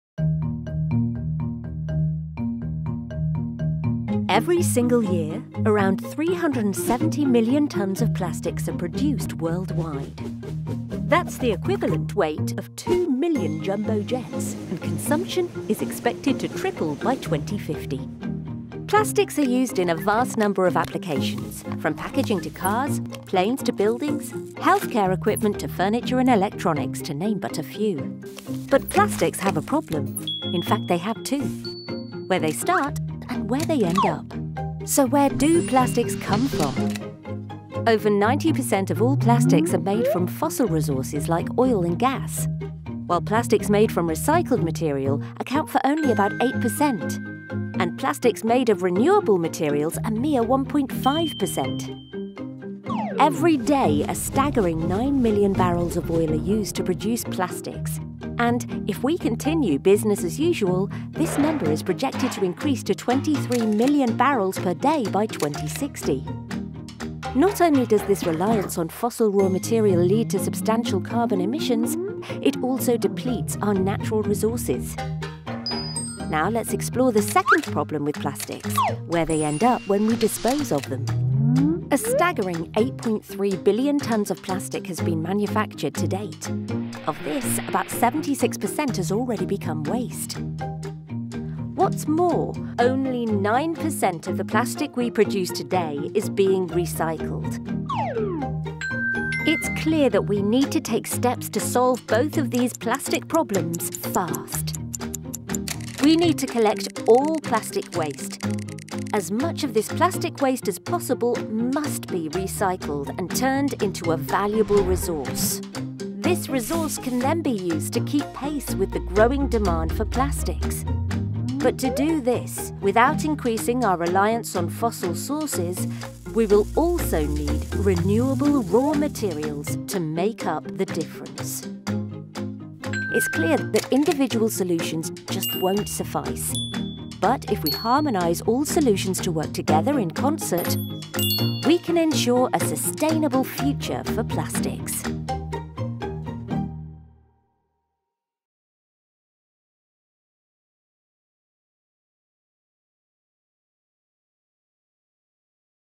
Plastics by numbers
Did you know how much plastics we produce each year? And what share of plastics is
recycled? We have plastics by the numbers for you - and what’s in for more sustainable
plastics in the future.